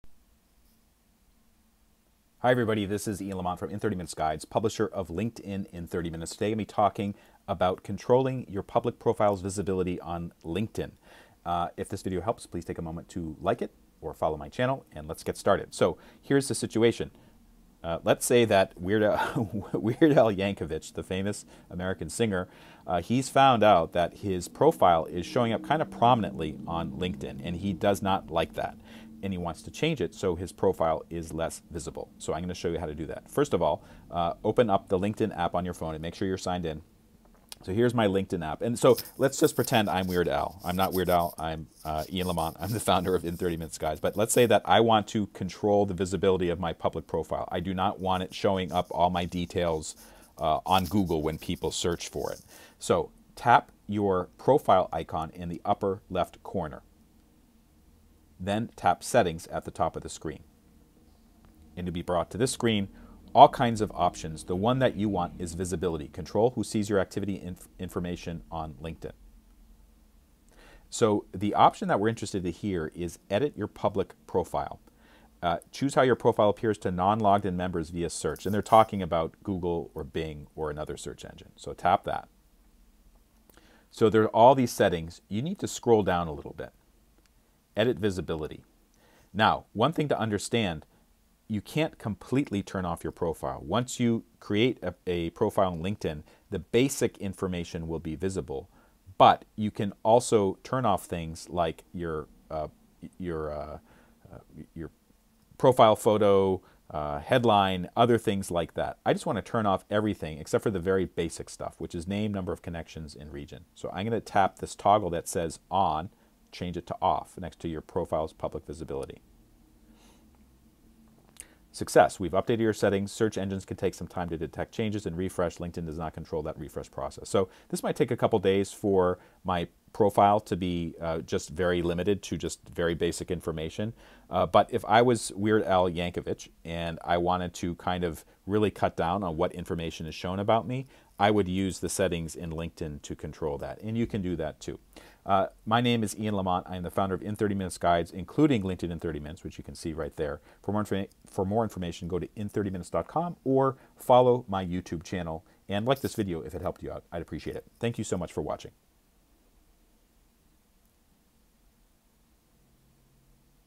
How to limit your LinkedIn public profile on Google and Bing with the LinkedIn iPhone app (2022)
iPhone 13 (unlocked) ➡️
M1 Macbook ➡️
Intel Macbooks/refurbished ➡️

From @in30minutes, how to change visibility settings on the LinkedIn app for iPhone. Narrator Ian Lamont is the founder of i30 Media Corporation, publisher of IN 30 MINUTES cheat sheets & guides.